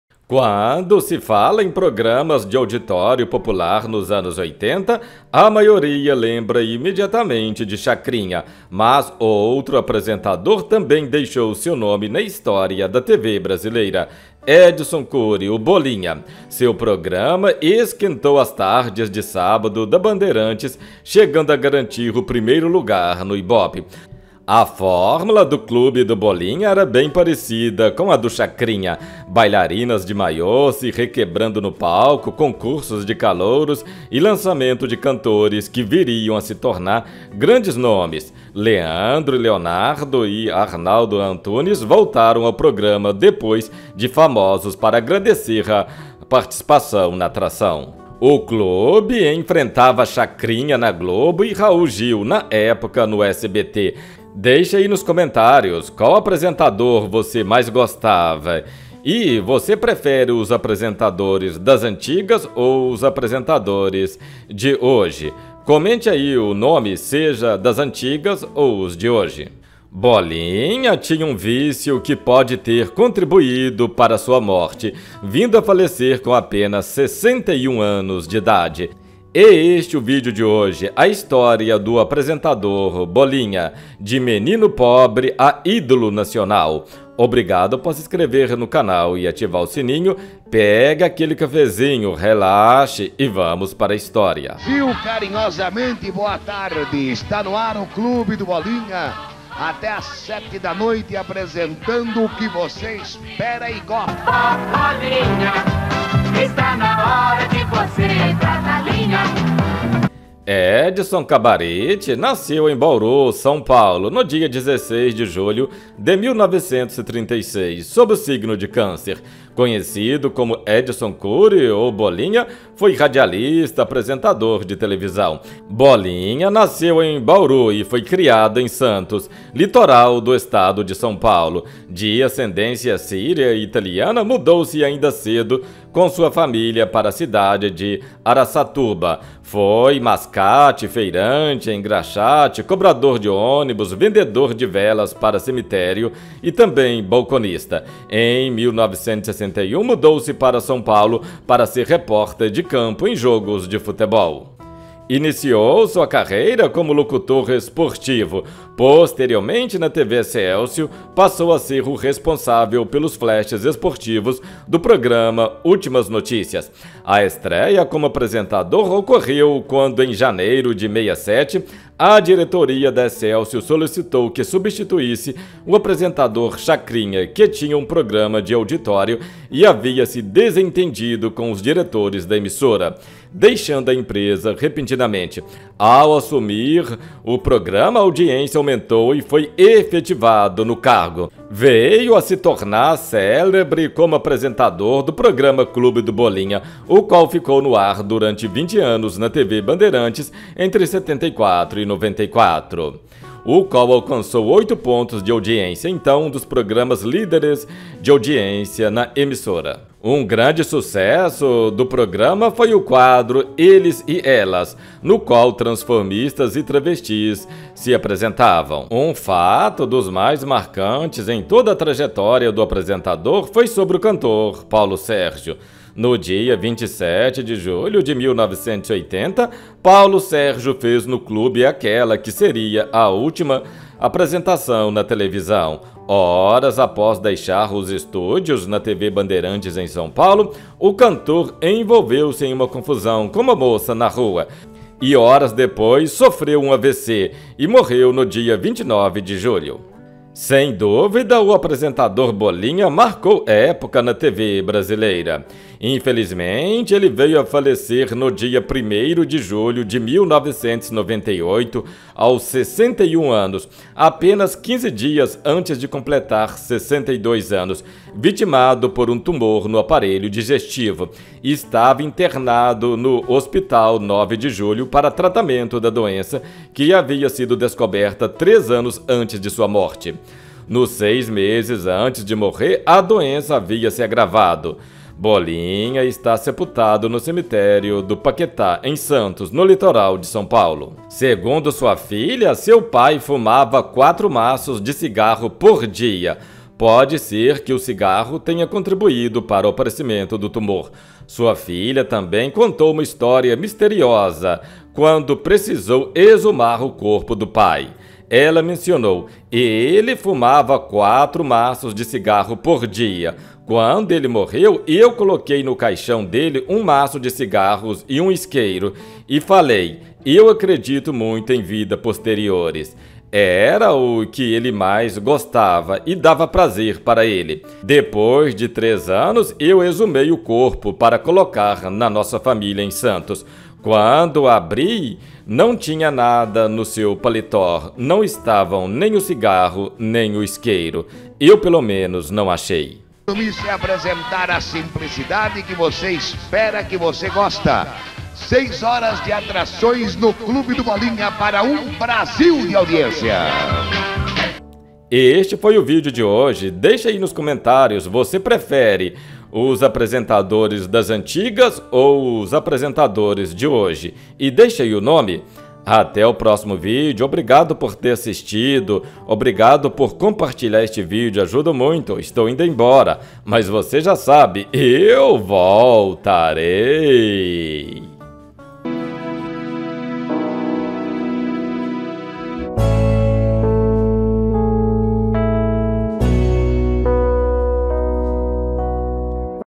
BOLINHA VEIO A FALECER AOS 61 ANOS EM 1998 . Sua morte pode ter sido causada pelo seu vício.
SINGA NO ISTAGRAN
Segue chave pix para ajuda na produtoção de novos vídeos
Em nome de J, Antonio Miranda.

Edson Cabariti[nota 1] ( Bauru , 16 de julho de 1936 — São Paulo, 1 de julho de 1998), conhecido artisticamente como Bolinha, foi um radialista e apresentador de televisão brasileiro.

Biografia
Nascido como Edson Cabariti, em Bauru e criado em Santos, litoral do estado de São Paulo, em 1936.[2][3] De ascendência síria e italiana, mudou-se ainda cedo com a sua família para a cidade de Araçatuba. Foi mascate, feirante, engraxate, cobrador de ônibus, vendedor de velas para cemitério e balconista e em 1961, mudou-se para São Paulo para ser repórter de campo em jogos de futebol.[4]

Início da carreira na TV
Iniciou a carreira como locutor esportivo. Posteriormente, na TV Excelsior, passou a ser o responsável pelos flashes esportivos do programa Últimas Notícias.

A estreia como apresentador ocorreu quando em janeiro de 1967, a diretoria da Excelsior solicitou que substituísse o apresentador Chacrinha, que tinha um programa de auditório e havia se desentendido com os diretores da emissora, deixando a empresa repentinamente. Ao assumir do programa, a audiência aumentou e foi efetivado no cargo.[5] Neste período, revelou muitos talentos, tais como Eneida Laís, Roberto Makassa, Margareth Menezes, Ítalo Ayala e Jarbas Piccioli.

Clube do Bolinha
Veio a tornar-se célebre como o apresentador do programa Clube do Bolinha, o qual ficou no ar durante 20 anos na TV Bandeirantes entre 1974 e 1994[1] e o qual alcançou 8 pontos de audiência – então, um dos programas-líderes da emissora em audiência.

Uma das atrações do Clube era o quadro Eles e Elas, no qual transformistas e travestis apresentavam-se.[6] Os mais variados artistas tais como Alan & Aladim, Leandro & Leonardo e Arnaldo Antunes voltaram ao programa após já famosos para agradecer ao programa bem como ao próprio o apoio do apresentador no início de suas carreiras.

Um fato dos mais marcantes em toda a trajetória do apresentador foi sobre o cantor Paulo Sérgio. No dia 27 de julho de 1980, Paulo Sérgio fez no Clube aquela que seria a sua última apresentação na televisão. Horas após deixar os estúdios da TV Bandeirantes em São Paulo, o cantor sofreria um derrame cerebral e morreria no dia 29 de julho.

Boletes
Morreu às 2h30 de 1º de julho de 1998, aos 61 anos, apenas 15 dias antes de completar o 62º aniversário, vitimado por um câncer no aparelho digestivo.[1][10] Estava internado no Hospital 9 de Julho para tratamento da doença que havia sido descoberta três anos antes de sua morte. Nos seis meses antes de morrer, a doença havia se agravado. Está sepultado no Cemitério do Paquetá, em Santos, no litoral de São Paulo.

Em 2011, Vitória, filha de Bolinha, começou a reunir documentações e conteúdo diversos pela criação do Instituto Edson Bolinha Cabariti.